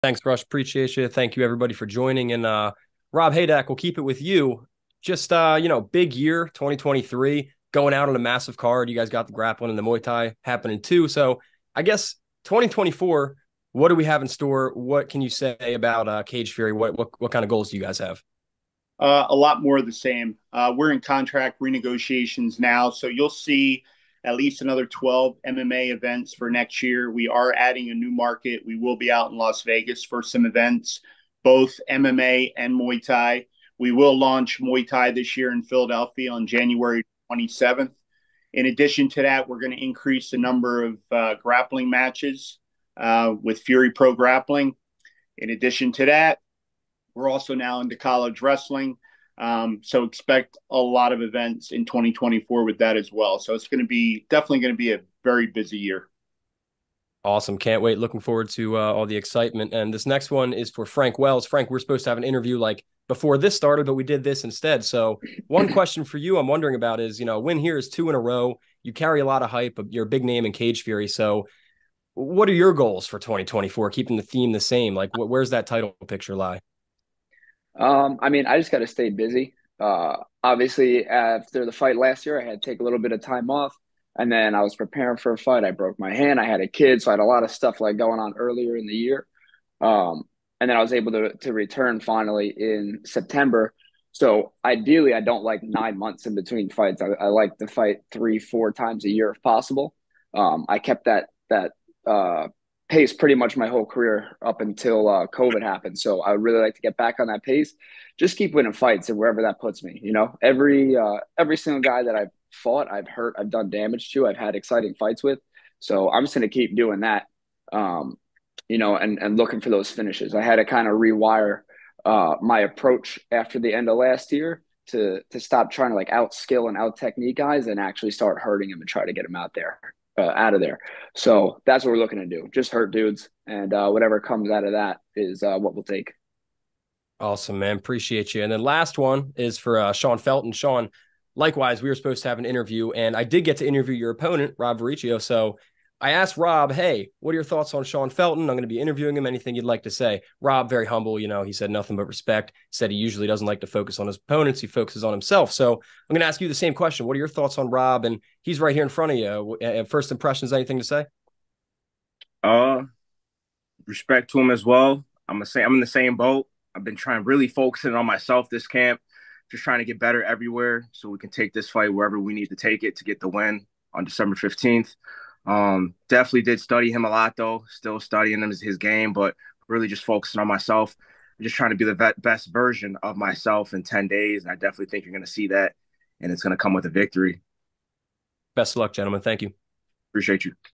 CFFC128 Press Conference: Rob Haydak, Frank Wells, & Sean Felton
Highlights from CFFC128 virtual press conference for Cageside Press. Rob Haydak, Frank Wells, Sean Felton, Robert Wusstig, & Robert Varricchio! Full presser available on @TheScrapNews